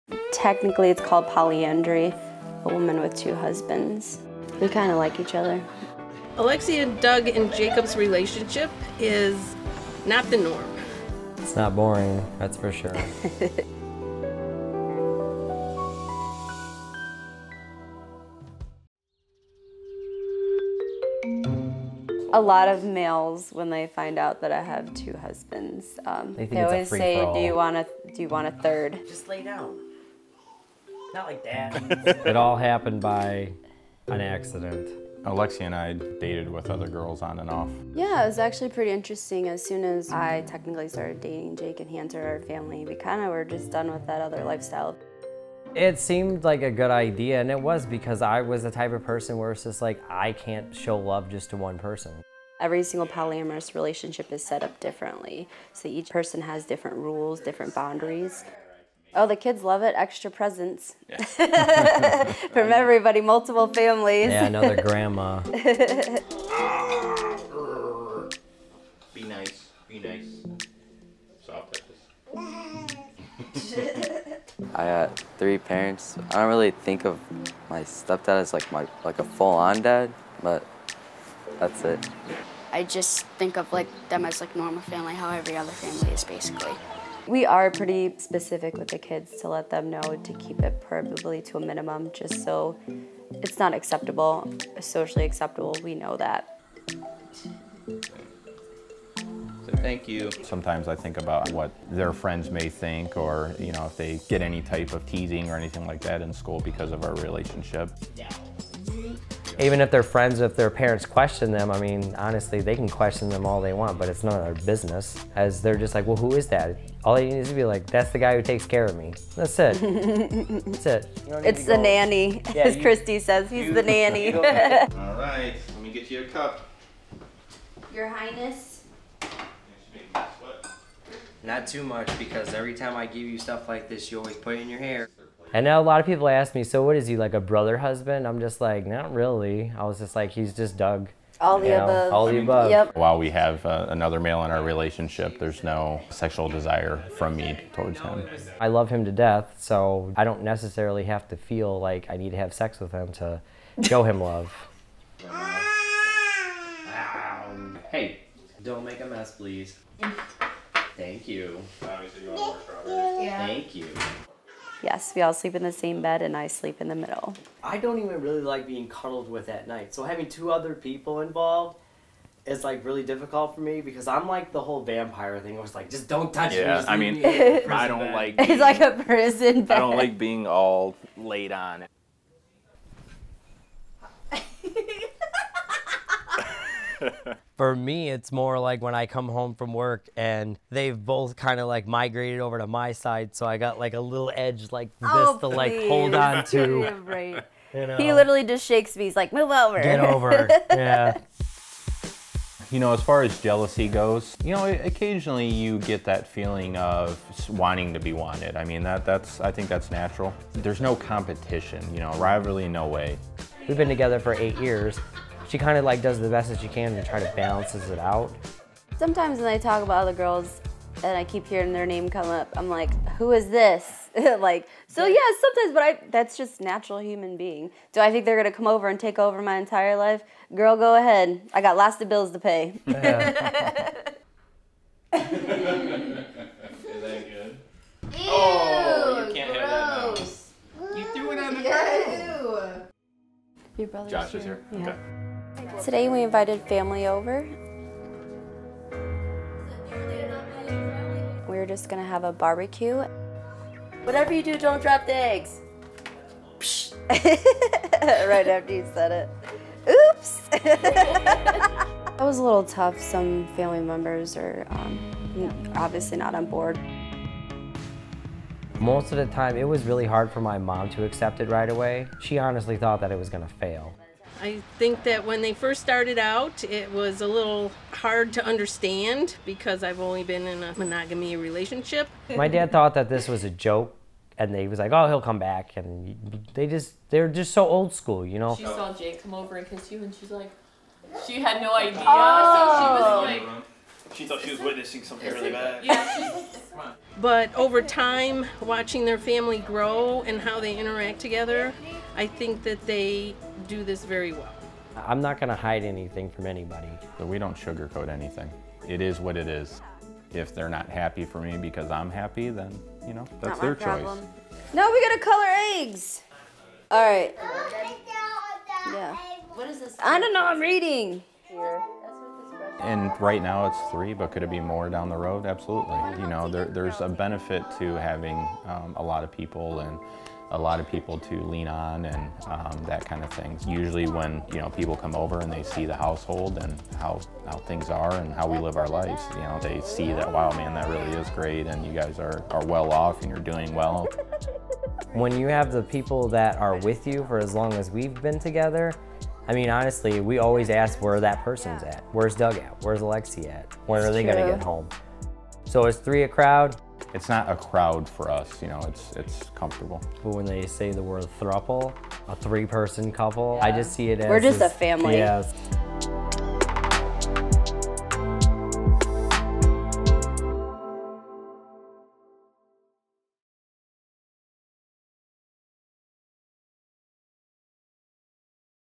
Raising My Kids With Two Husbands | EXTREME LOVE
A POLYAMOROUS thruple are opening up about their relationship to prove that three isn’t a crowd. Alexia Shrout lives with her two husbands, Doug Shrout, and Jacob Shrout-Mchaffie and their three children in Maumee, Ohio. Alexia, who works as a professional body piercer, was in an open relationship with Doug when she met Jacob. Doug and Alexia have been together fifteen years, opening up their relationship to Jake eight years ago; and in July 2016 the trio had a ceremony to mark their commitment to one another. The unique family are raising their three children, Skyler, 14, Donovan, 11 and one-year-old baby girl Teegan. The two sons are biologically Doug’s, however, to avoid favouritism the triad have not disclosed the biological father of their daughter.

Video Credits:
Videographer / director: Grayson Beras
Producer: Shannon Lane, Ruby Coote
Editor: Shiona Penrake